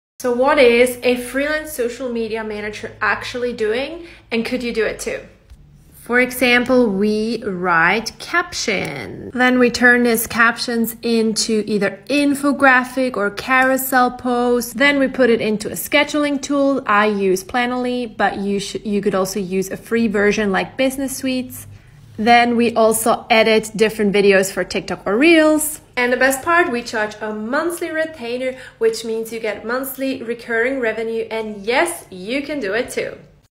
What Does A Social Media Manager Do?
What does a social media manager actually do, the tasks, tools and how do we get paid?